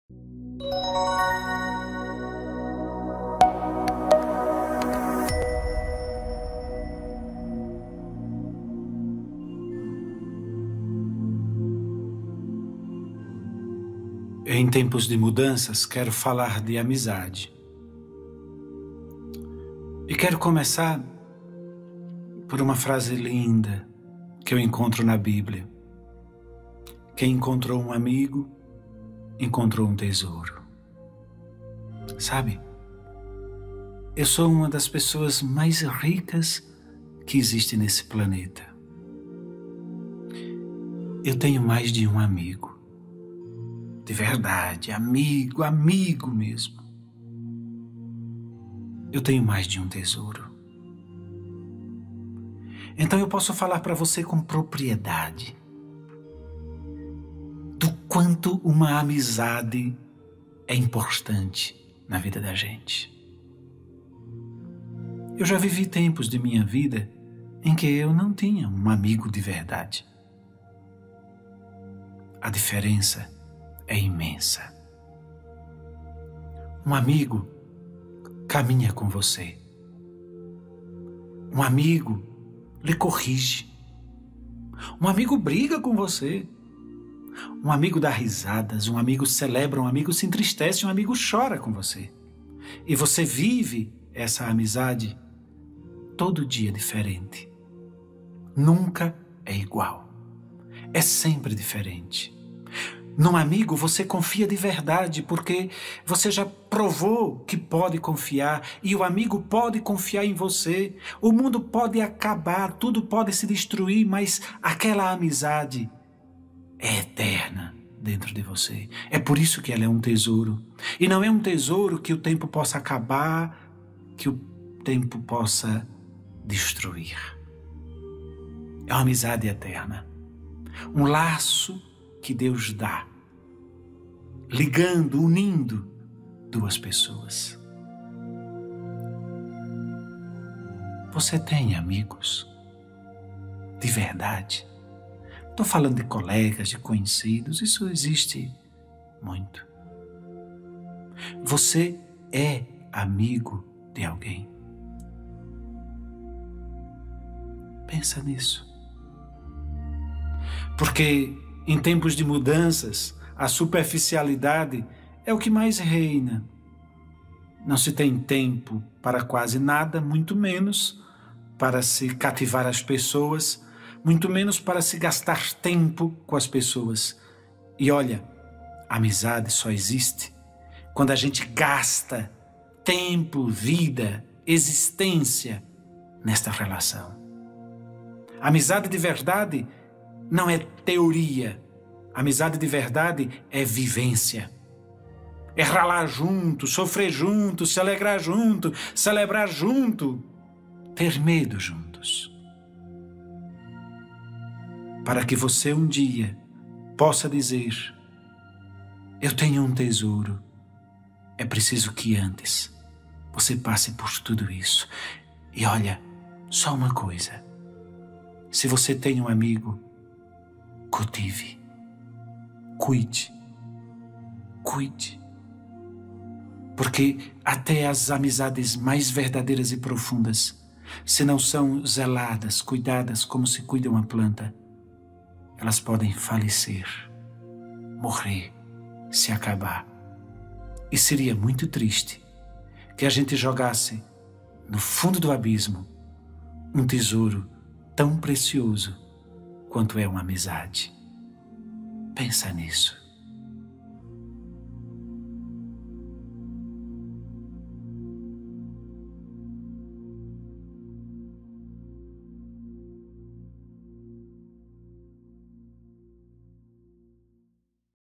A Amizade
Nesse vídeo o Pe. Pedro Cunha fala sobre a Amizade em tempos de mudanças